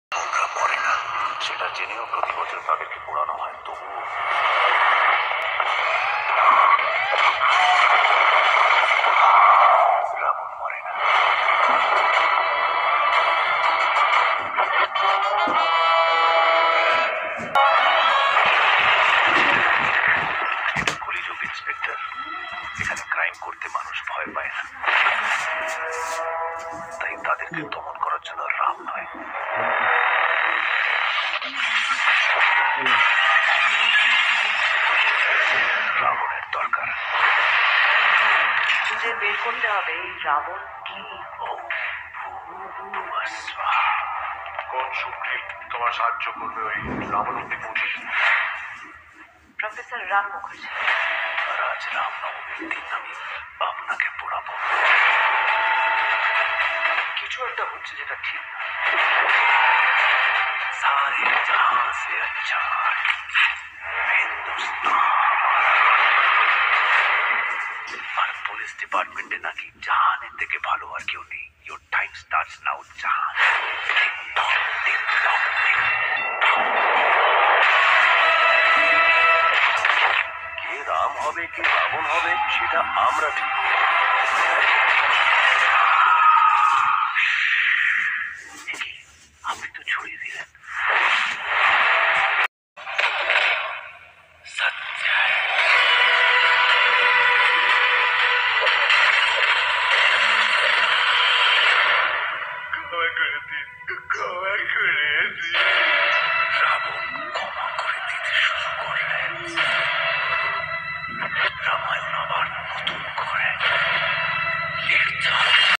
Bengali Superstar Jeet Raavan Drawing//Raavan Jeet Drawing // Raavan Drawing Tutorial
Today we teach raavan movie Jeet Drawing. Bangla movie raavan drawing video.